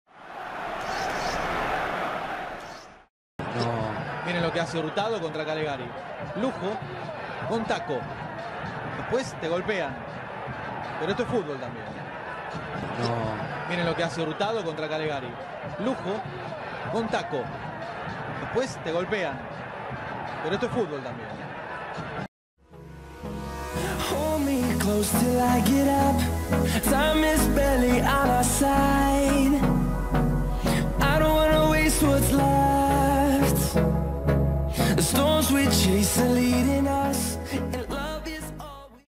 ¡Amazing! La locura de regate de este jugador en la Copa Argentina ⚽ REVIEW ⚽ 2019
🔸 El delantero venezolano Jan Hurtado no para de crecer y en el partido de en el campo de Newell's se atrevió con este recurso al alcance de muy pocos.